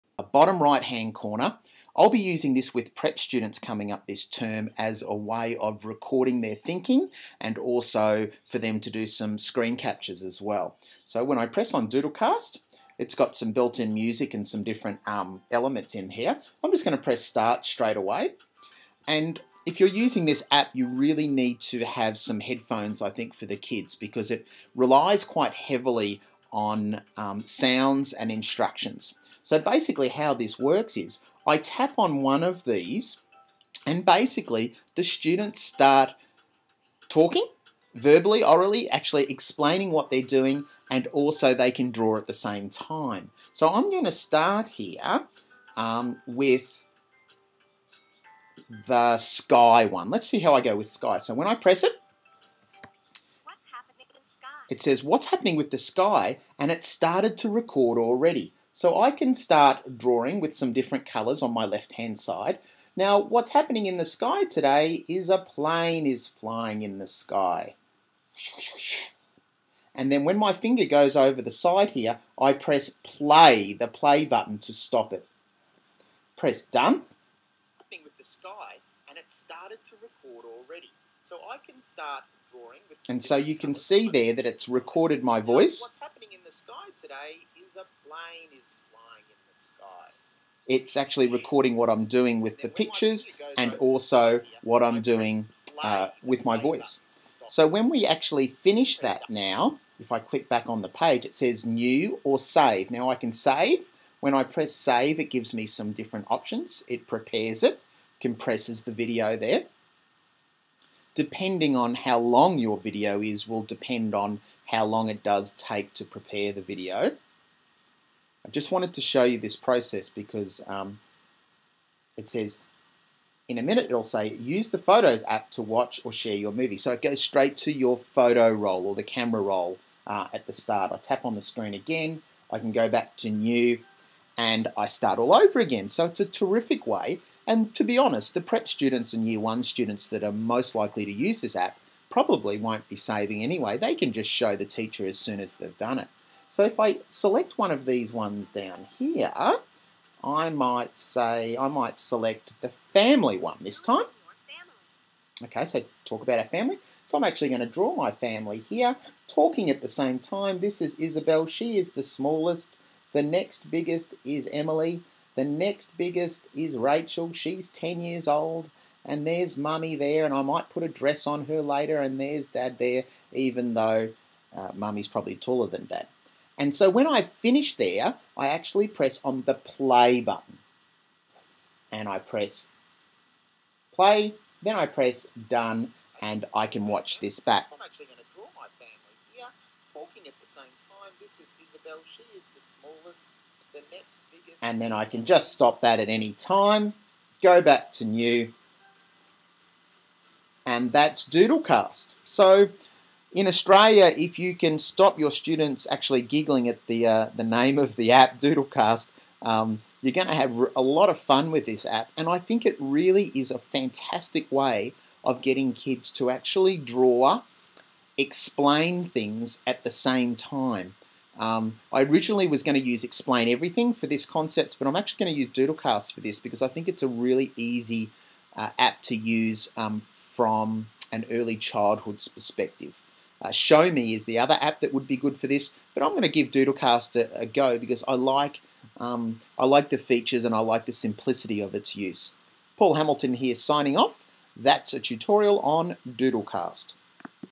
Doodlecast App tutorial
Doodlecast tutorial. Paul Hamilton explains how Doodlecast can be used in classrooms as a explaining thinking tool.